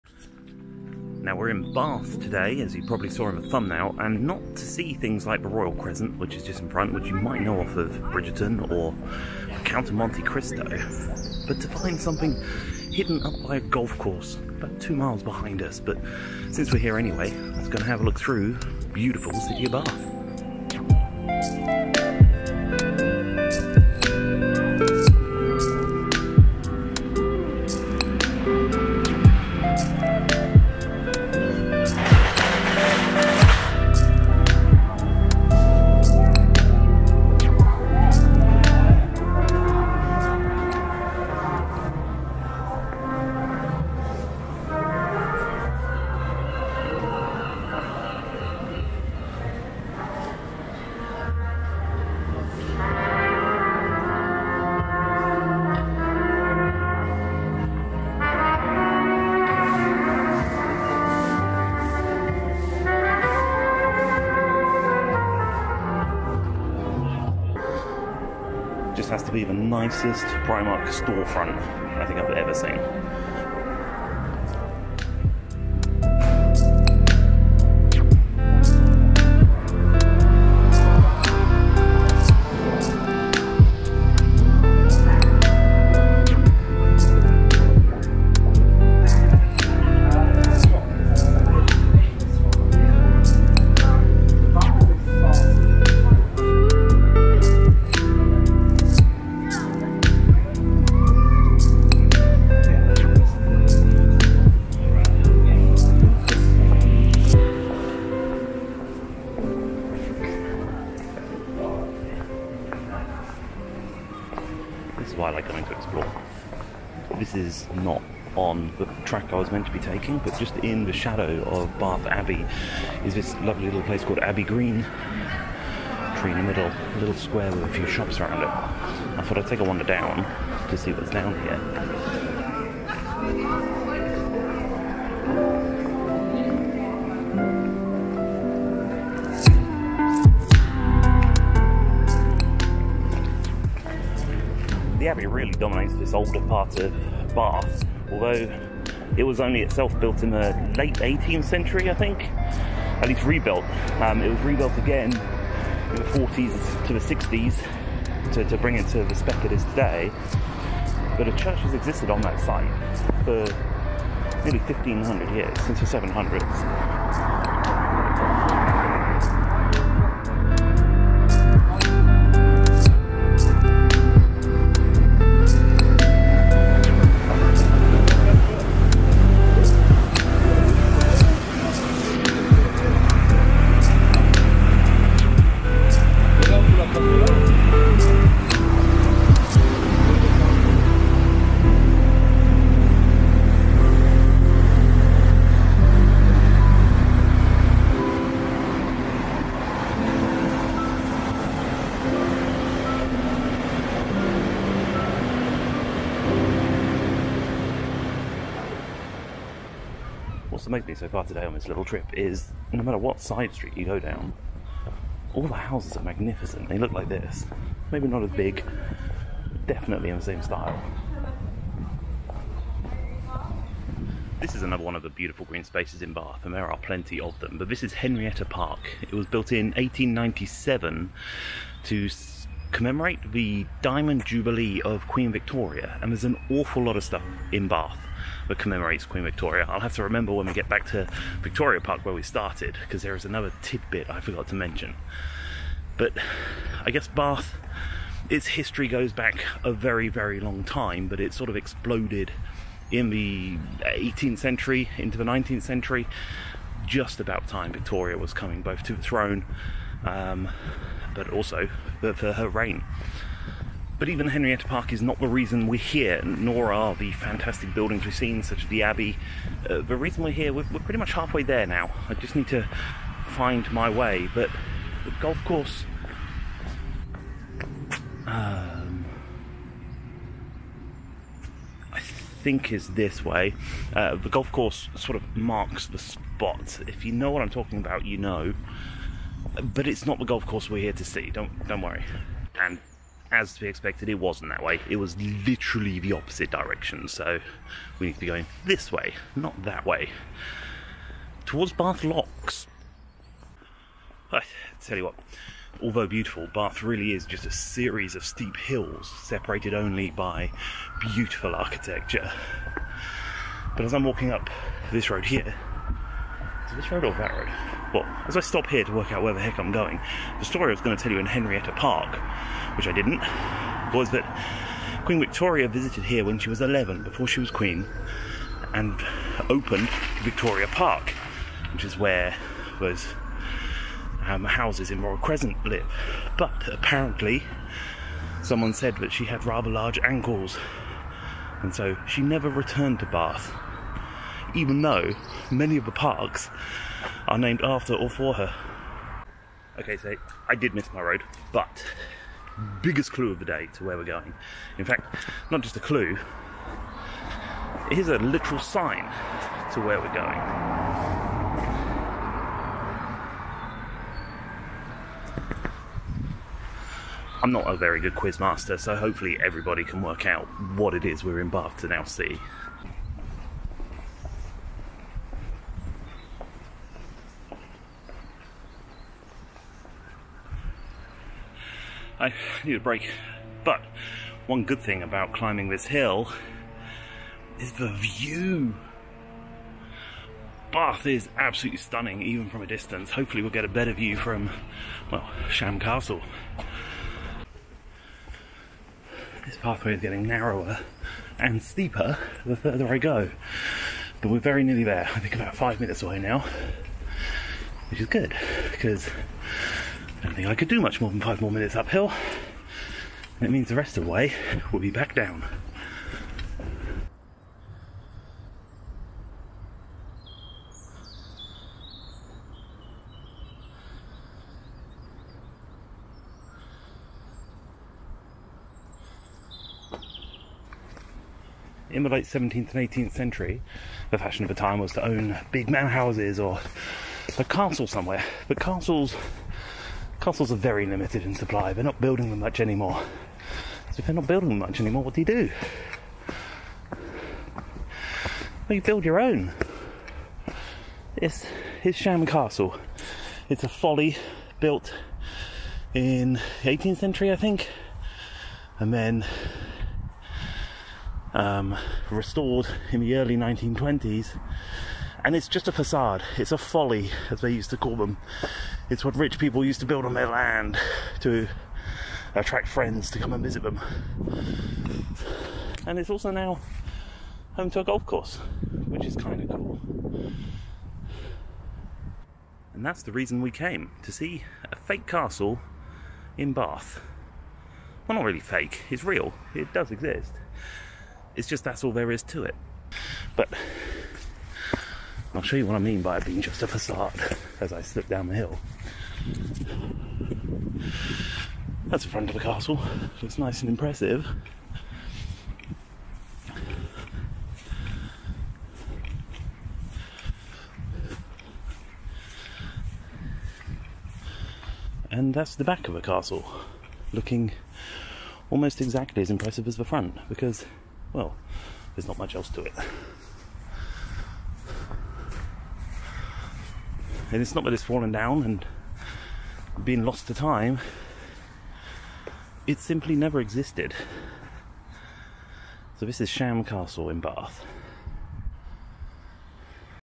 A different way to spend a day in Bath
In this video, I visit the beautiful city of Bath, and although we walk past Victoria Crescent, St Michael's Church, Bath Abbey, The Roman Baths, Pulteney Bridge and Henrietta Park, none of these are our destination.

Instead, we're heading our way up to Sham Castle, to see a folly from the 18th Century.

So what can you see in one day in Bath? Is it a walkable city? What are some of the sights? Should you visit Bath? What to see on a daytrip to Bath? Well, hopefully in this Vlog you'll get a sense of how great Bath, UK, is to visit.